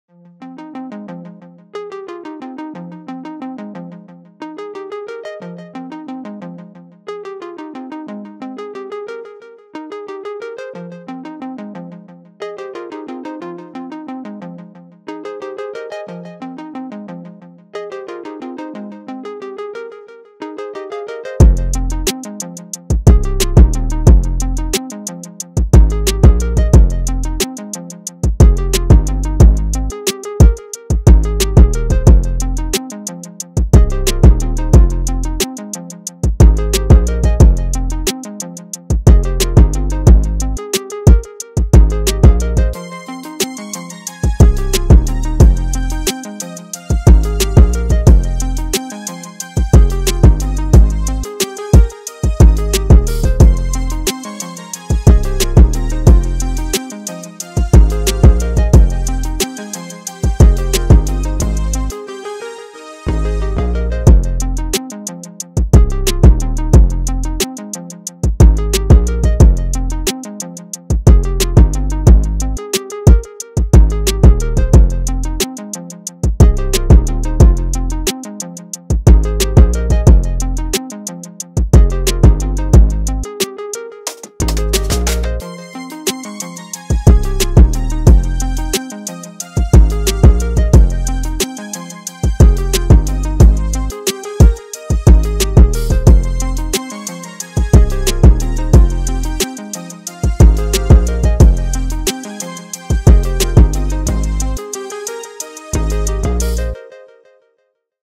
lvl 1
every melody i make sounds like it should be in a video game